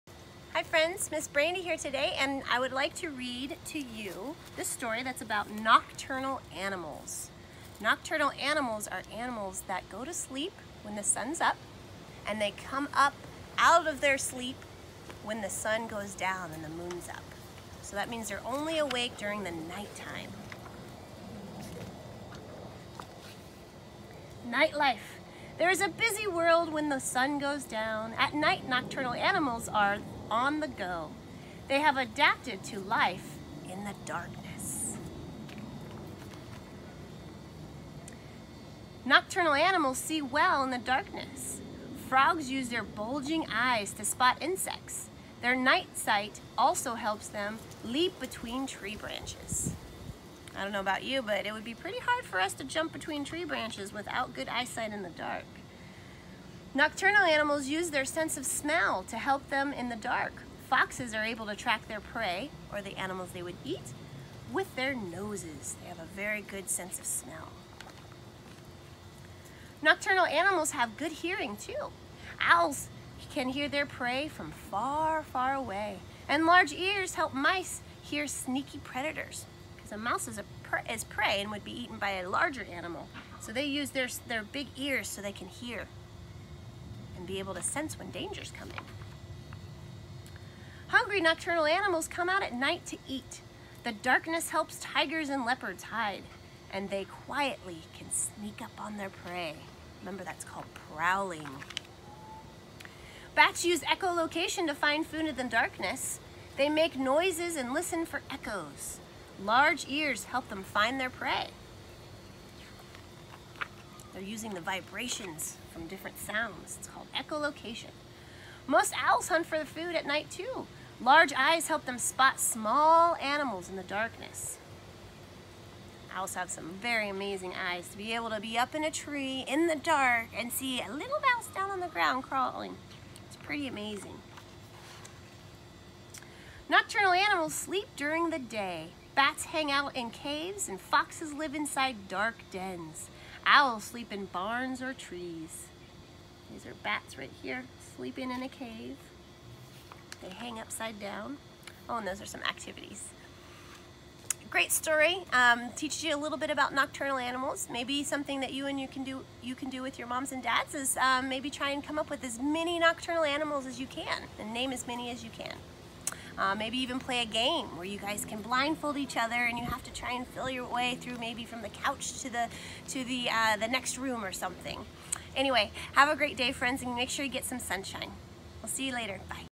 Nocturnal Animals read by Mrs Brandy 🦇🦉🐸
Nocturnal Animals by Abbie Dunne read by Mrs Brandy 🦇🦉🐸